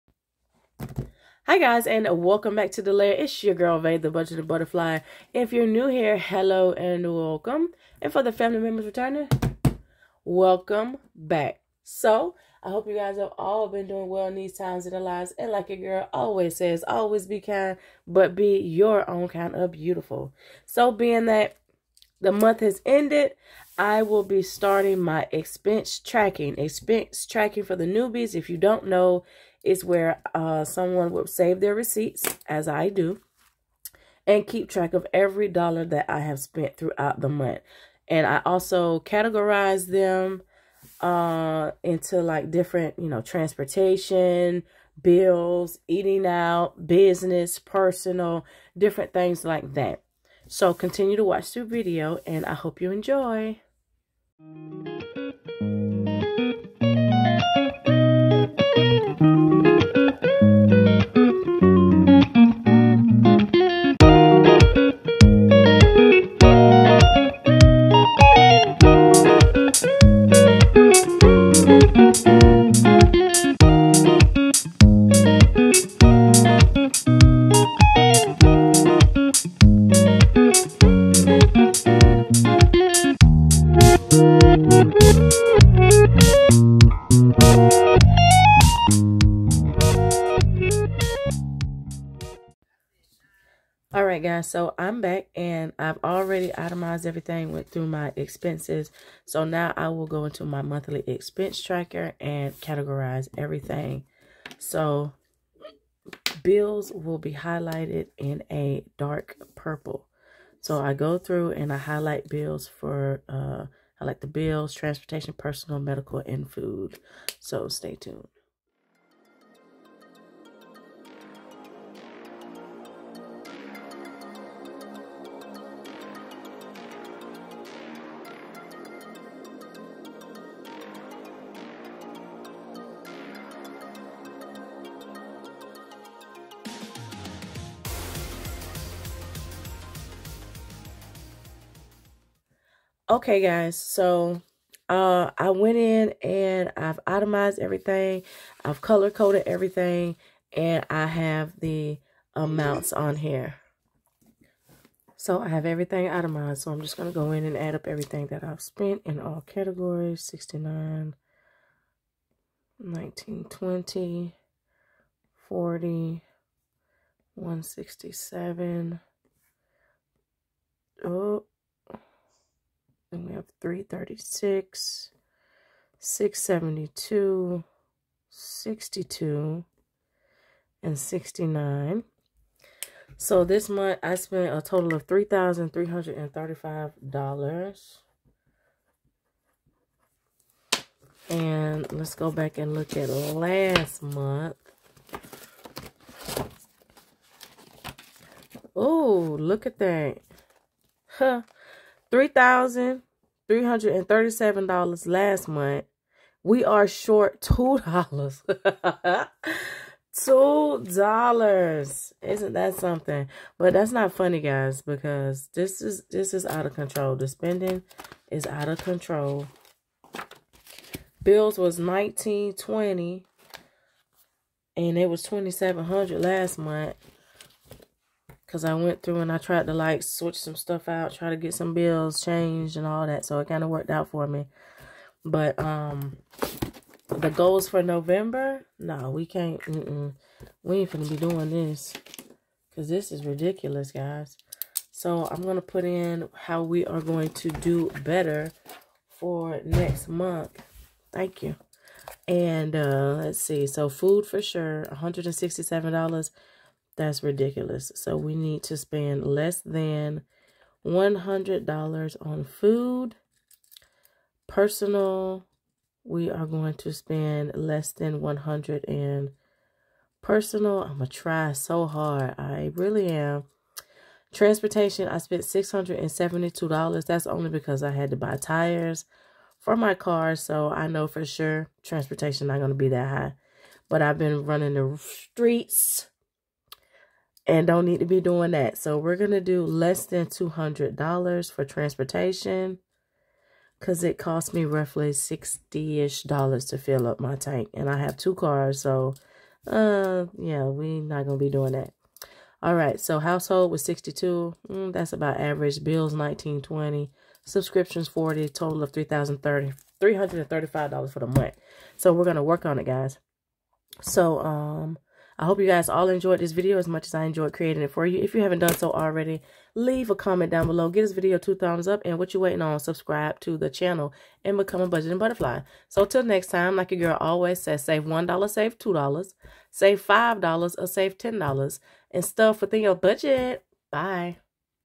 OCTOBER EXPENSE TRACKING WHAT DID I SPEND !?
Hey guys, this is just a brief overview of my category. With what I spend money in every month? This way I can track and see where I need to improve and spending .